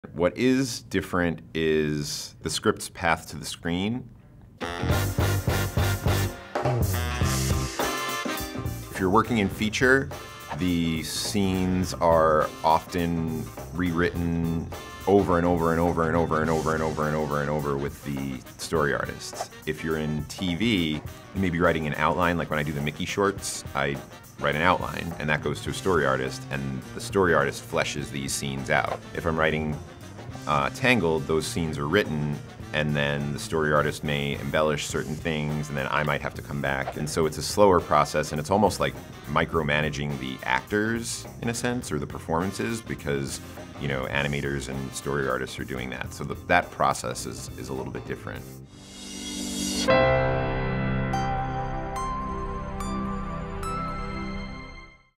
What is the Difference Between Writing for TV and Movies? A Seasoned Writer Explains
The path to the screen can be very different depending on the medium you're writing for, such as animation, live-action, TV or feature-length content. But what does the process look like behind the scenes? We sat down with Disney writer Ricky Roxburgh, who specifically writes for Disney Animation Television but has also written features, to get an inside look into how the writing process varies.